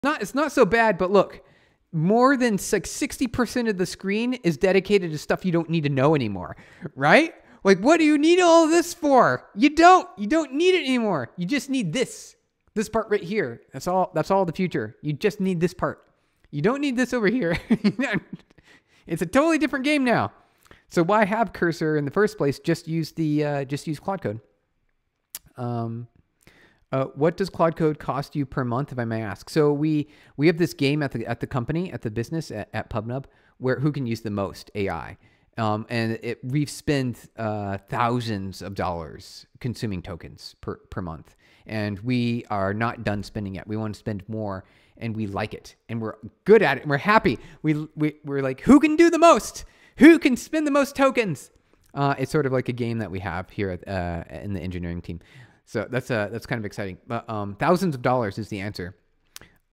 Cloud Code vs. Traditional Dev Tools Costs
It is not that bad, but really, more than sixty percent of the screen shows things you do not need anymore. Why would you need all this extra stuff? You do not.

You just need this main part right here. That is what matters now. The rest is not important.

Things have changed a lot, so why keep using Cursor? Just switch to Cloud Code. Do you know how much Cloud Code costs every month?

At PubNub, we have this game to see who can use the most AI. We spend thousands of dollars each month on tokens, and we are not planning to stop. We actually want to spend more, and we have fun with it. Our engineering team always tries to top each other.

So yes, we spend thousands each month, and we enjoy it.